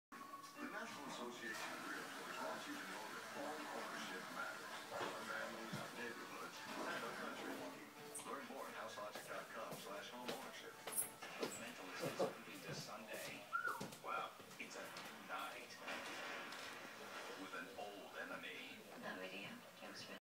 Newest pups
Jade and Chipper pups. 2 girls and a boy.